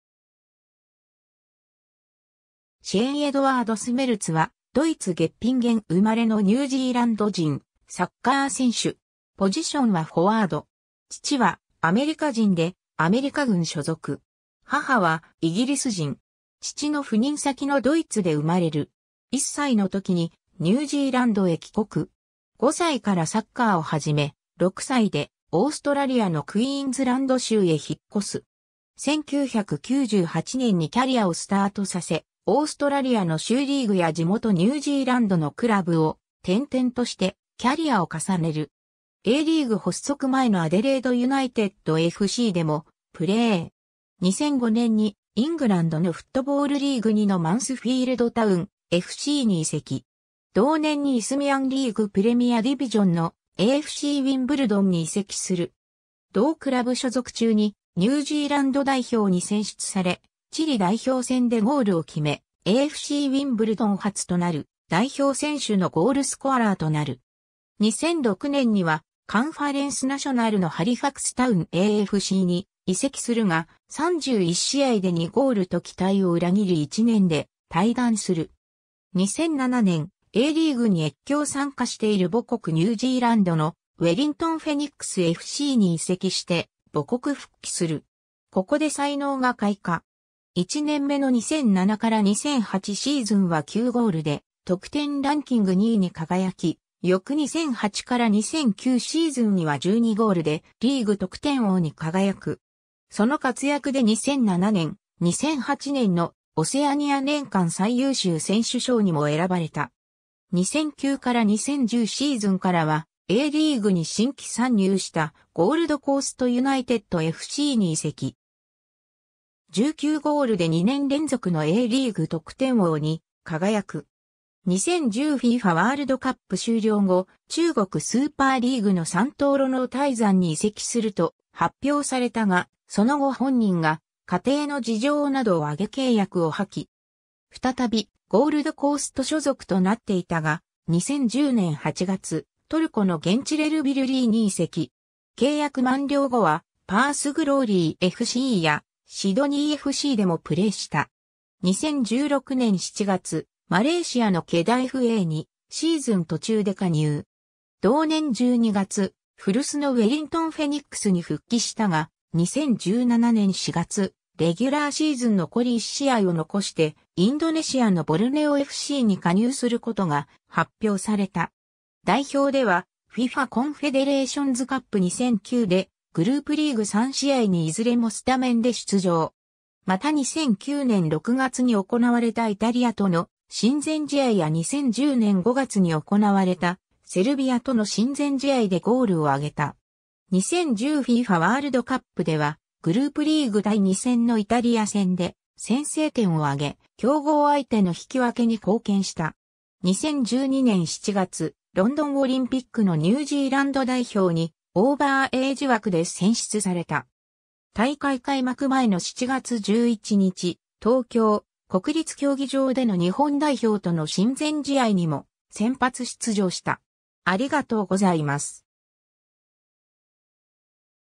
シェーン・スメルツ
ブリスベン・ストライカーズFCの選手
アデレード・シティFCの選手
アデレード・ユナイテッドの選手
マンスフィールド・タウンFCの選手
シェーン・エドワード・スメルツ（Shane Edward Smeltz, 1981年9月29日 - ）は、ドイツ・ゲッピンゲン生まれのニュージーランド人サッカー選手。
 父はアメリカ人でアメリカ軍所属。
母はイギリス人。
父の赴任先のドイツで生まれる。
1歳の時にニュージーランドへ帰国。
5歳からサッカーを始め、6歳でオーストラリアのクイーンズランド州へ引越す。
 1998年にキャリアをスタートさせ、オーストラリアの州リーグや地元ニュージーランドのクラブを転々としてキャリアを重ねる。
Aリーグ発足前のアデレード・ユナイテッドFCでもプレー。
 2005年にイングランドのフットボールリーグ2(4部相当)のマンスフィールド・タウンFCに移籍。
同年にイスミアンリーグ・プレミアディヴィジョン(7部相当)のAFCウィンブルドンに移籍する。
同クラブ所属中にニュージーランド代表に選出され、チリ代表戦でゴールを決め、AFCウィンブルドン初となる代表選手のゴールスコアラーとなる。
2006年にはカンファレンス・ナショナル(5部相当)のハリファクス・タウンAFCに移籍するが、31試合で2ゴールと期待を裏切り1年で退団する。
 2007年、Aリーグに越境参加している母国ニュージーランドのウェリントン・フェニックスFCに移籍して母国復帰する。
ここで才能が開花。
1年目の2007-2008シーズンは9ゴールで得点ランキング2位に輝き、翌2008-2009シーズンには12ゴールでリーグ得点王に輝く。
その活躍で2007年、2008年のオセアニア年間最優秀選手賞にも選ばれた。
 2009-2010シーズンからはAリーグに新規参入したゴールドコースト・ユナイテッドFCに移籍。
19ゴールで2年連続のAリーグ得点王に輝く。
 2010 FIFAワールドカップ終了後、中国スーパーリーグの山東魯能泰山に移籍すると発表されたが、その後本人が家庭の事情などを挙げ契約を破棄。
再びゴールドコースト所属となっていたが、2010年8月、トルコのゲンチレルビルリイに移籍。
契約満了後はパース・グローリーFCやシドニーFCでもプレーした。
 2016年7月、マレーシアのケダFAにシーズン途中で加入。
同年12月、古巣のウェリントン・フェニックスに復帰した が、2017年4月、レギュラーシーズン残り1試合を残してインドネシアのボルネオFCに加入することが発表された。
 代表ではFIFAコンフェデレーションズカップ2009でグループリーグ3試合にいずれもスタメンで出場。
また2009年6月に行われたイタリアとの親善試合や2010年5月に行われたセルビアとの親善試合でゴールを挙げた。
2010 FIFAワールドカップではグループリーグ第2戦のイタリア戦で先制点を挙げ、強豪相手の引き分けに貢献した。
 2012年7月、ロンドンオリンピックのニュージーランド代表にオーバーエイジ枠で選出された。
大会開幕前の7月11日、東京・国立競技場での日本代表との親善試合にも先発出場した。